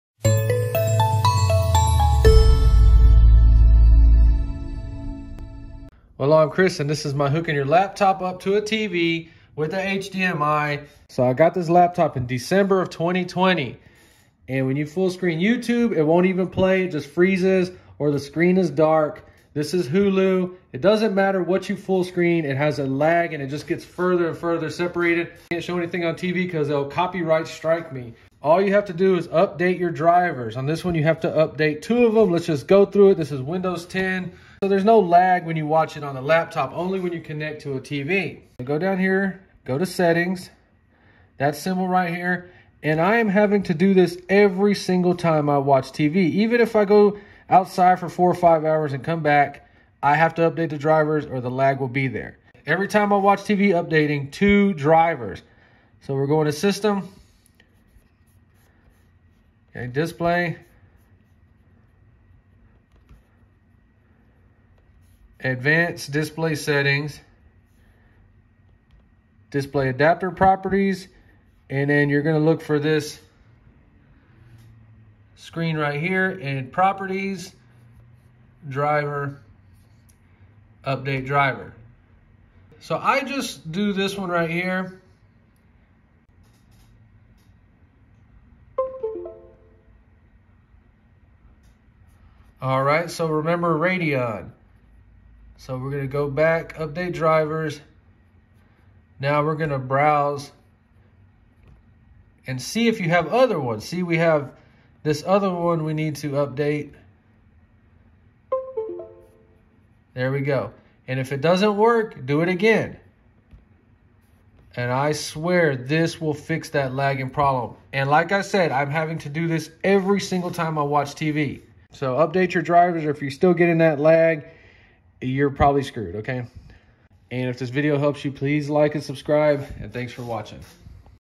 LAPTOP connected to TV with HDMI LAG FIX hulu youtube fullscreen windows 10
i bought a NEW HP OMEN laptop DECEMBER 2020 and it lagged so bad when i hooked it up to a TV with HDMI it would not even play youtube fullscreen.. THIS IS HOW I FIXED IT